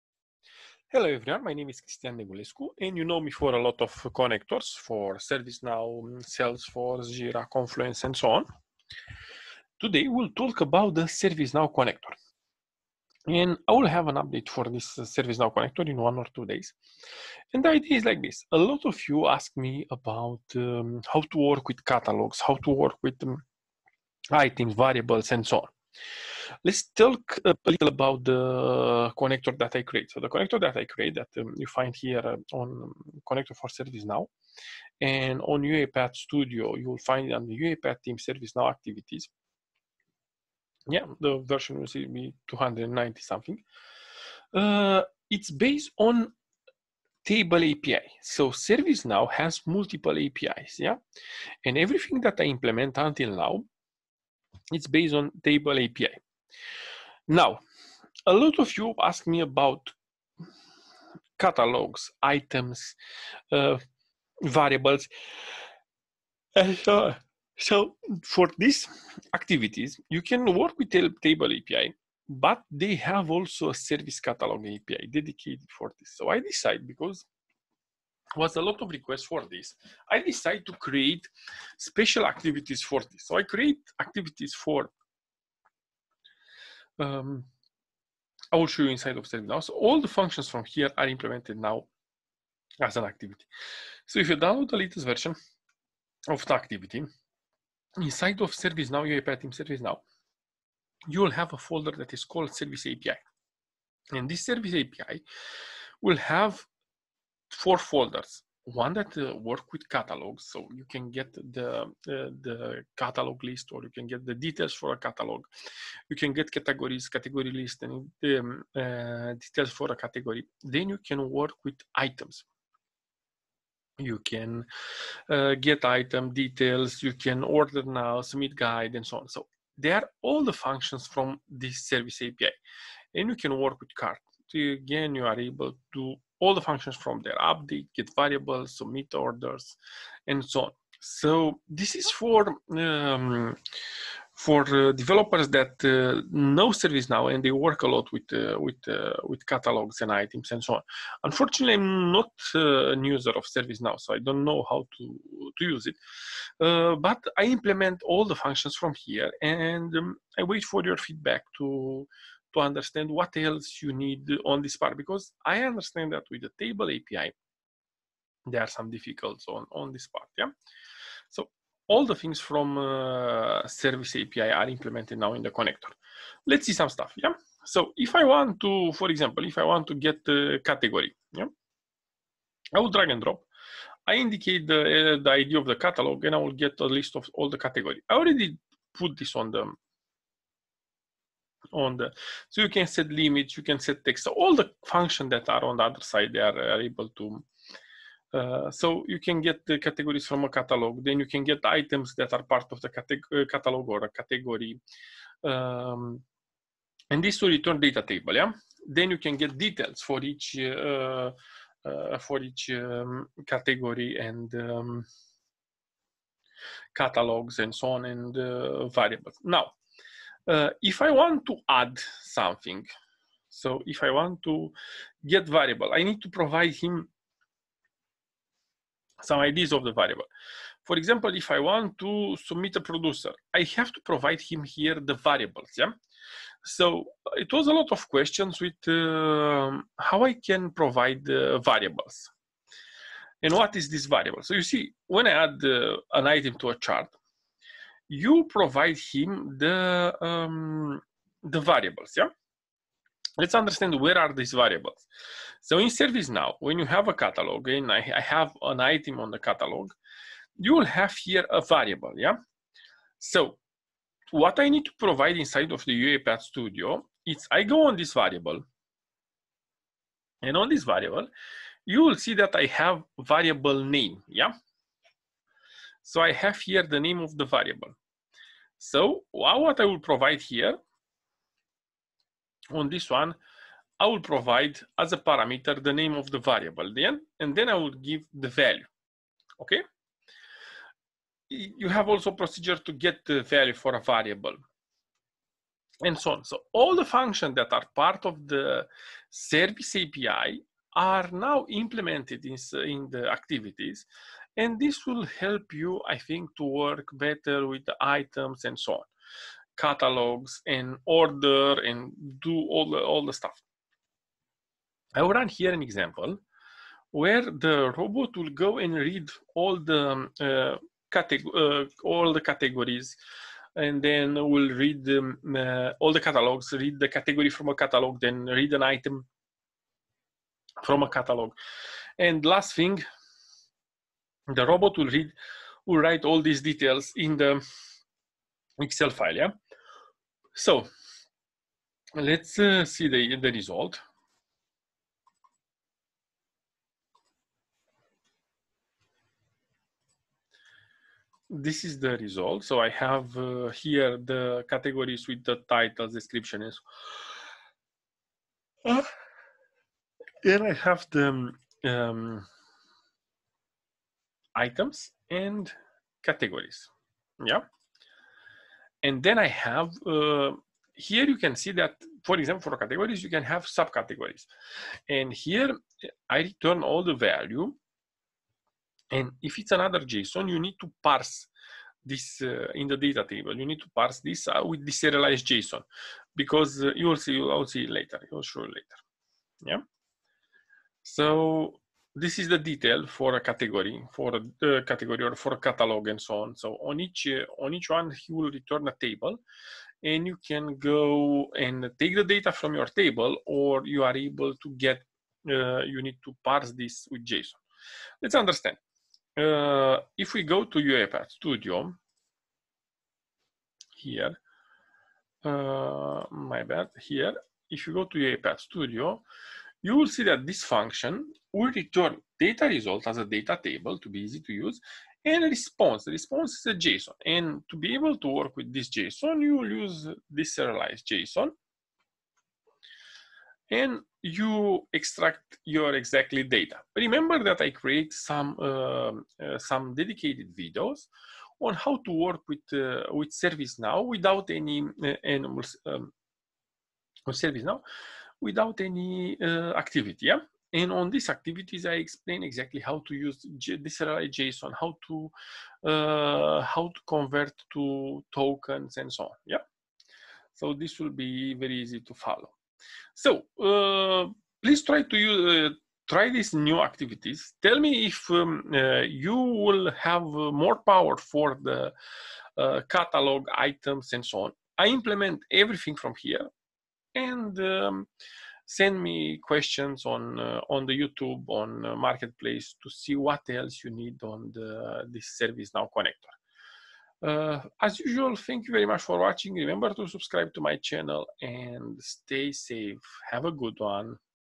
Unlocking the Secrets of ServiceNow's Service Catalog API
Service Catalog API helps you to work with Catalogs, Categories, Items, and Cart.

0:00 Intro
0:47 REST API commands from serviceNow Website
1:35 New Activies in UiPath
2:20 Catalogs
4:15 Variables
5:15 Run the process